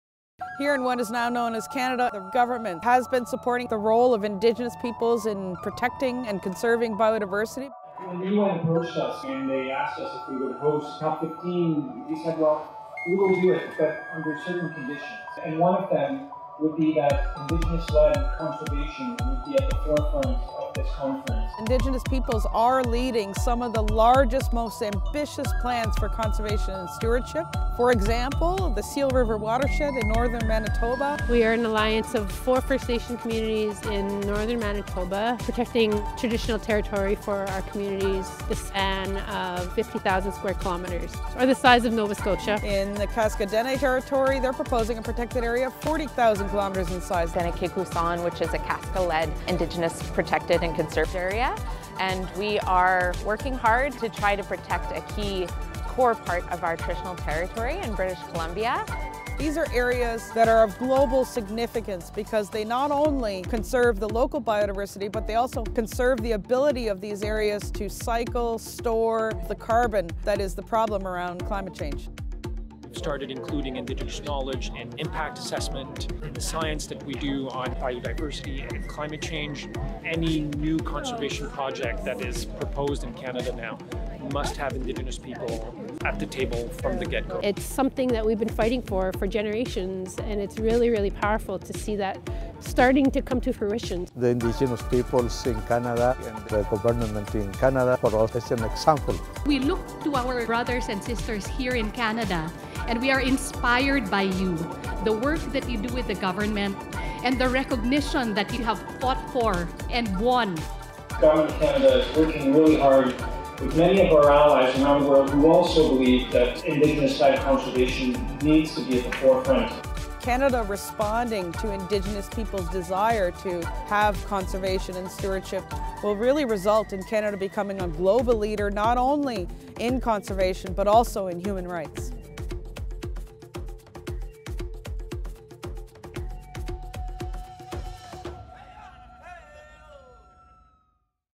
The Indigenous Leadership Initiative & COP15: Canada Leading the Way
We left the UN Biodiversity Summit this year with a full heart and hope for the future: in the Village and beyond, it was inspiring to see the impact of Indigenous-led conservation around the world  – and all that’s possible for the future. At COP15, Environment and Climate Change Minister Guilbeault shared that “any new conservation project that is proposed in Canada now must have Indigenous People at the table, from the get-go.” This approach – one that recognizes Indigenous leadership, rights, and knowledge –  can help Canada become a global leader, not only in biodiversity protection, but also in human rights.